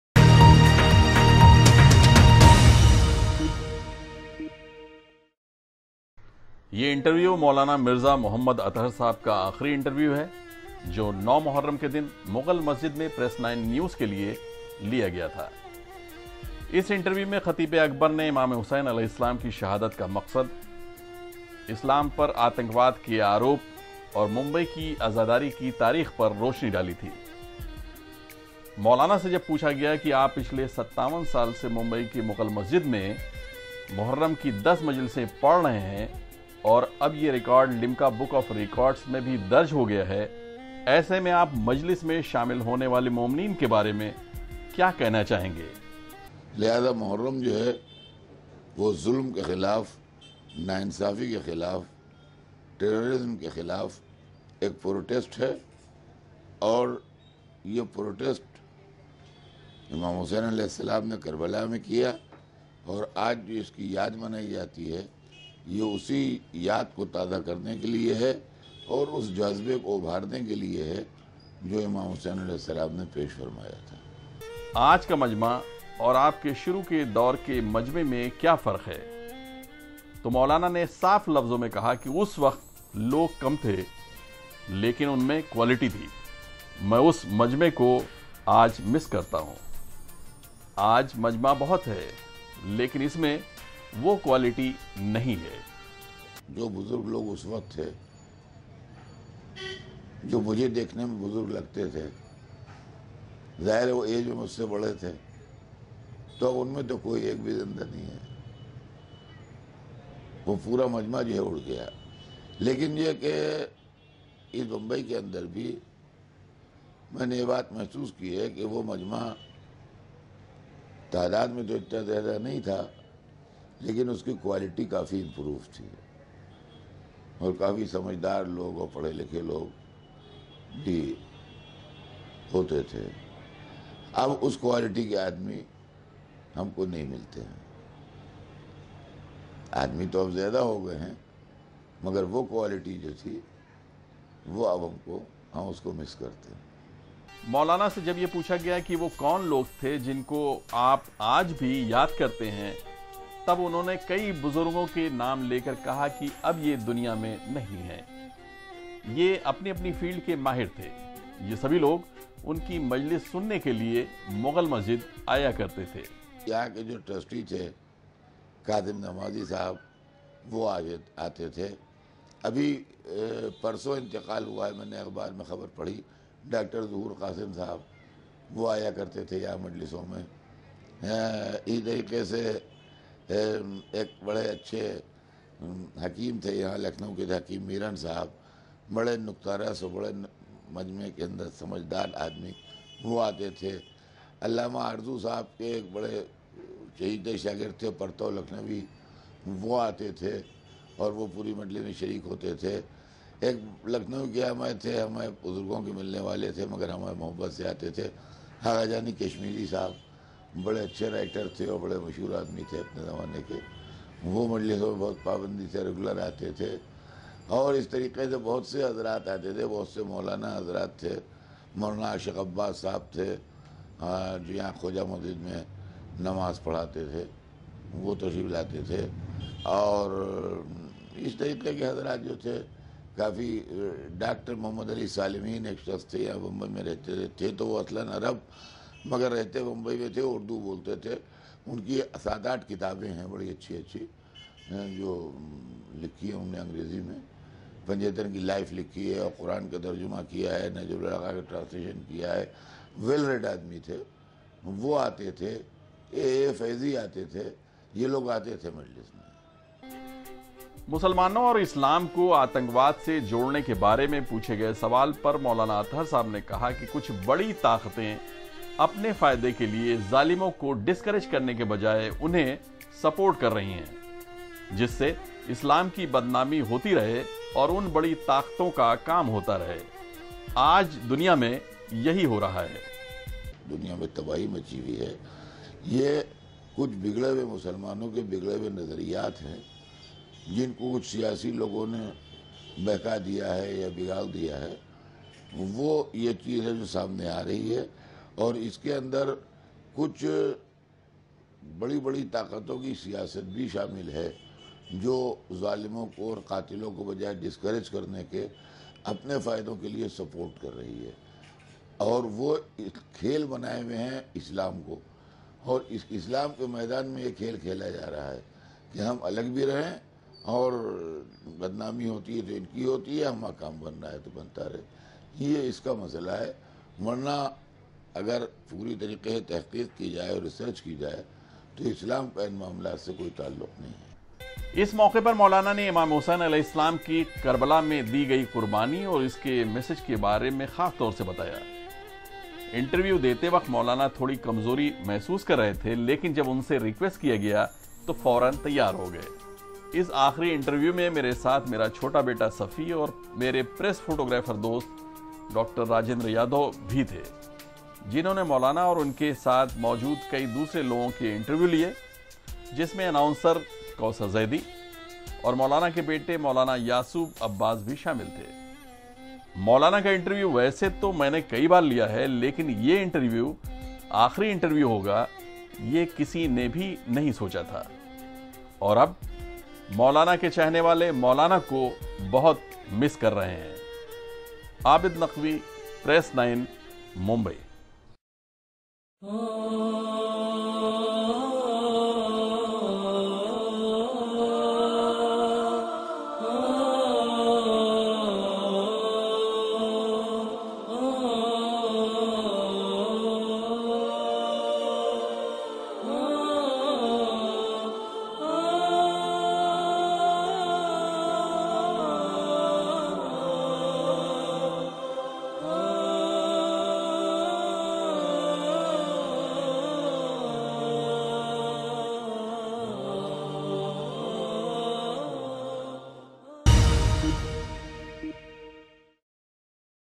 Last Interview of Maulana Mirza Mohammed Athar Saheb Qibla
Maulana Mirza Mohammed Athar Sb. recited 57 yrs continuously Majlis in Mughal Masjid, Mumbai. This is a record which was registered by Limca book of World Record. He was a good orator and his style of speech is very famous among youth because of his language. He used light language for children and youths.